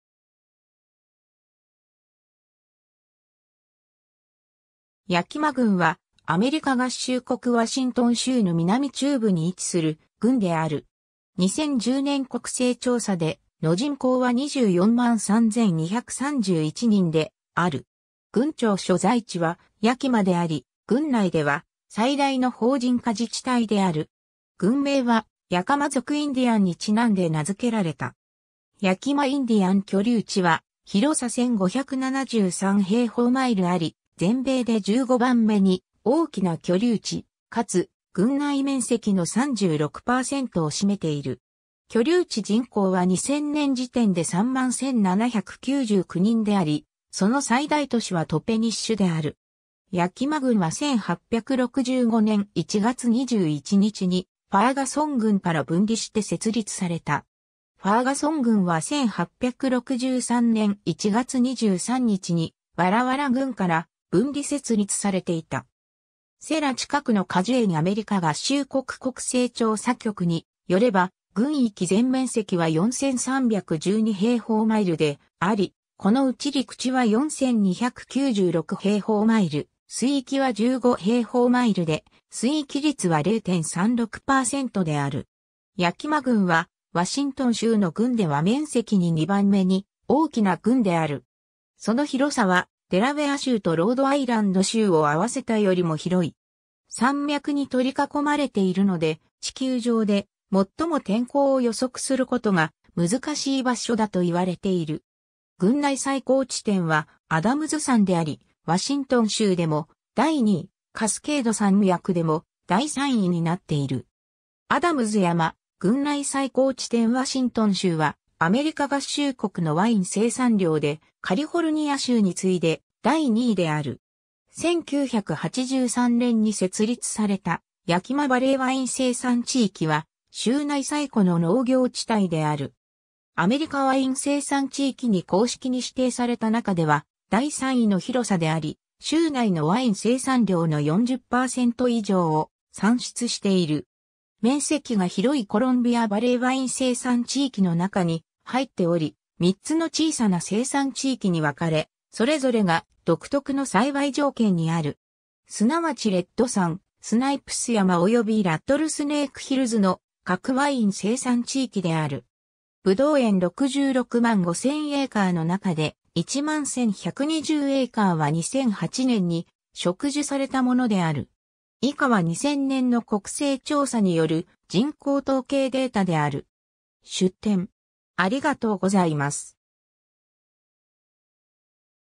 ヤキマ郡 (ワシントン州)
ヤキマ郡（英: Yakima County）は、アメリカ合衆国ワシントン州の南中部に位置する郡である。
2010年国勢調査での人口は243,231人である。
郡庁所在地はヤキマであり、郡内では最大の法人化自治体である。
郡名はヤカマ族インディアンに因んで名付けられた。
 ヤキマインディアン居留地は広さ1,573平方マイル (4,074 km2) あり、全米で15番目に大きな居留地、かつ郡内面積の36%を占めている。
居留地人口は2000年時点で31,799人であり、その最大都市はトペニッシュである。
 ヤキマ郡は1865年1月21日にファーガソン郡（現在は廃郡）から分離して設立された。
ファーガソン郡は1863年1月23日にワラワラ郡から分離設立されていた。
 セラ近くの果樹園 アメリカ合衆国国勢調査局に拠れば、郡域全面積は4,312平方マイル (11,167km2)であり、このうち陸地は4,296平方マイル (11,127 km2)、水域は15平方マイル (40 km2)で水域率は0.36%である。
ヤキマ郡はワシントン州の郡では面積に2番目に大きな郡である。
その広さはデラウェア州とロードアイランド州を合わせたよりも広い。
山脈に取り囲まれているので、地球上で最も天候を予測することが難しい場所だと言われている。
郡内最高地点はアダムズ山であり、ワシントン州でも第2位、カスケード山脈でも第3位になっている。
 アダムズ山、郡内最高地点 ワシントン州はアメリカ合衆国のワイン生産量でカリフォルニア州に次いで第2位である。
1983年に設立されたヤキマバレー・ワイン生産地域は州内最古の農業地帯である。
アメリカ・ワイン生産地域に公式に指定された中では第3位の広さであり、州内のワイン生産量の40%以上を産出している。
 面積が広いコロンビアバレー・ワイン生産地域の中に入っており、3つの小さな生産地域に分かれ、それぞれが独特の栽培条件にある。
すなわちレッド山、スナイプス山およびラットルスネークヒルズの各ワイン生産地域である。
ブドウ園665,000エーカー (269,000 ha) の中で、11,120エーカー (4,500 ha) は2008年に植樹されたものである。
 出典